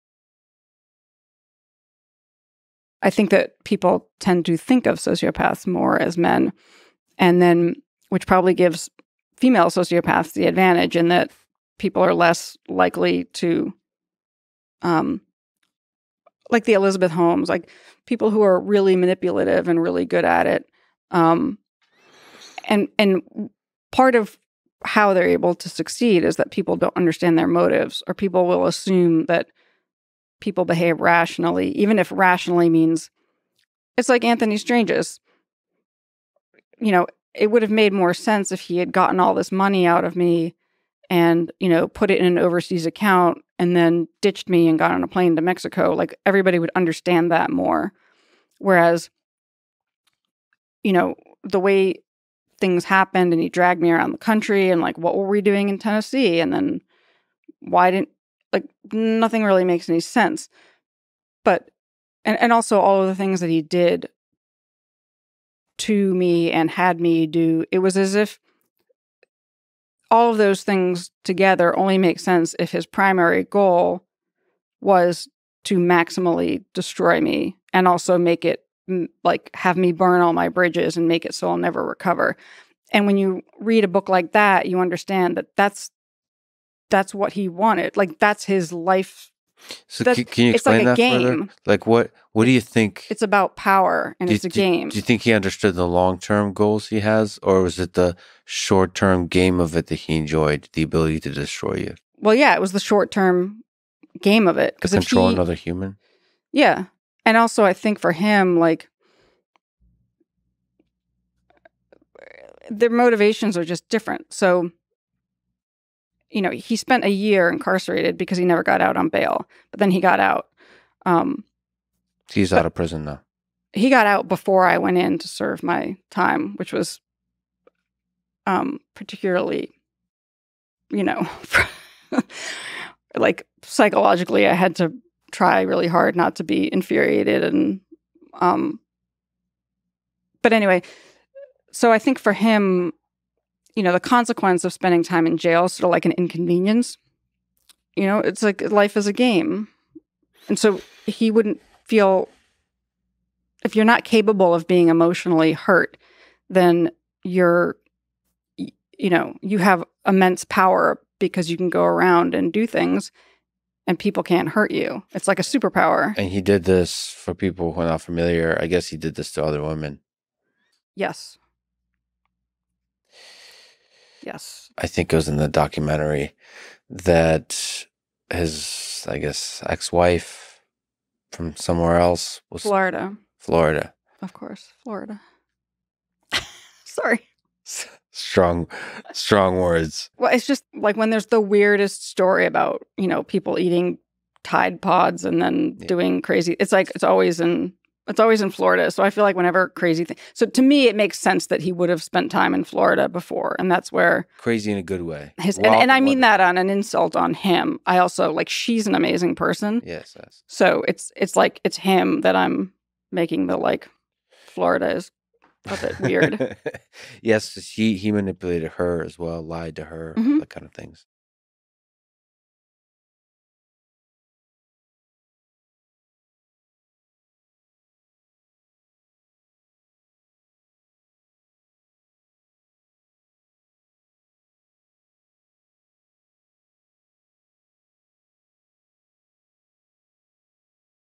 How to spot a sociopath | Sarma Melngailis and Lex Fridman
- Notion to get up to $1000 off team plan

GUEST BIO:
Sarma Melngailis is a chef and restauranteur who was the subject of the Netflix documentary Bad Vegan: Fame, Fraud, Fugitives.